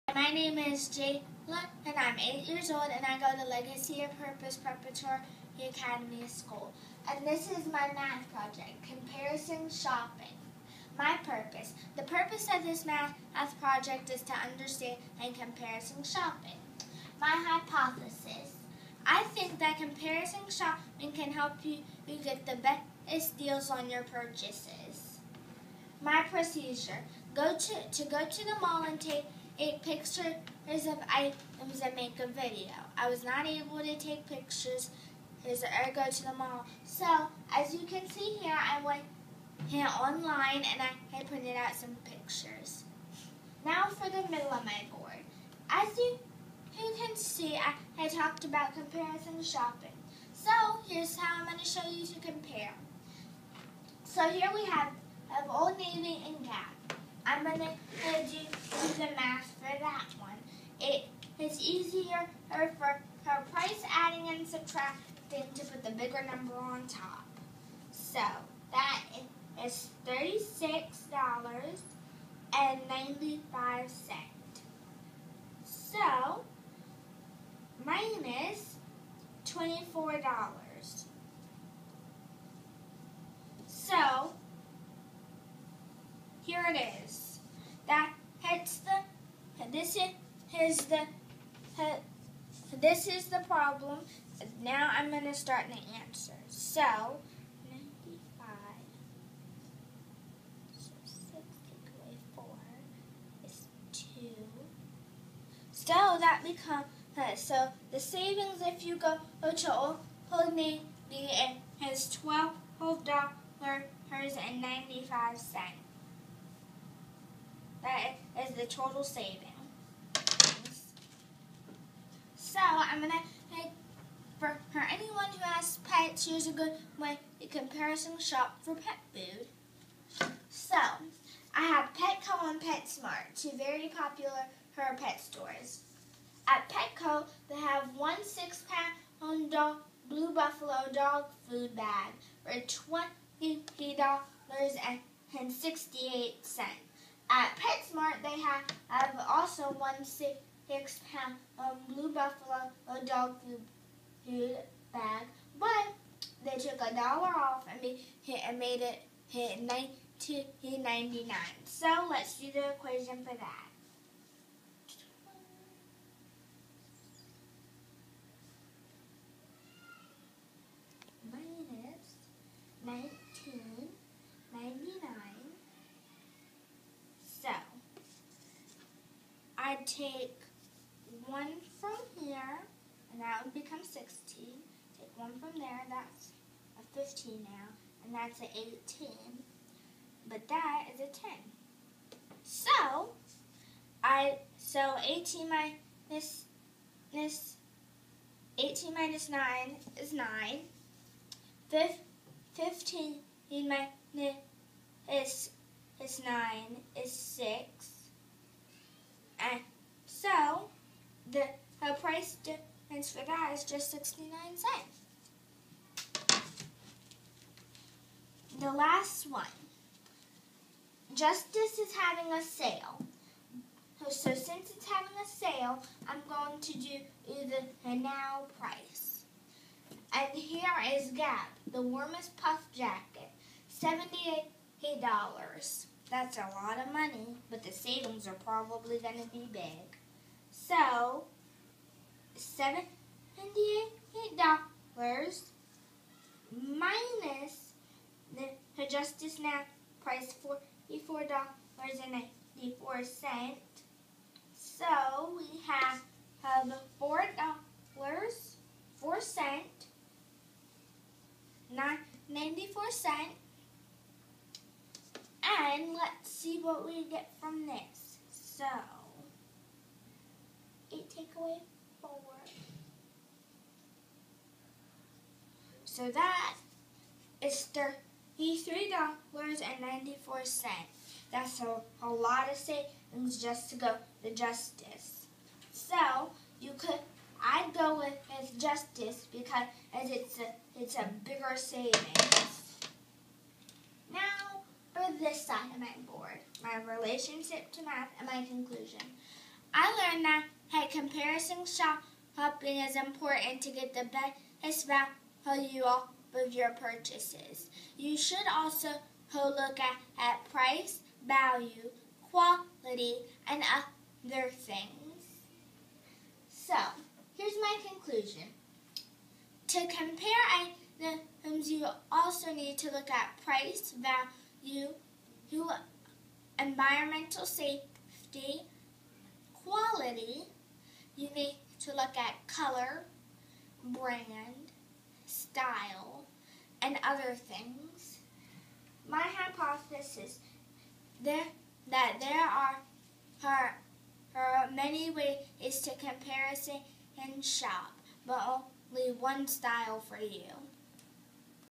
Jaylin's Math Fair Project
Student at Legacy of Purpose Preparatory Academy prepares for her Math Fair Presentation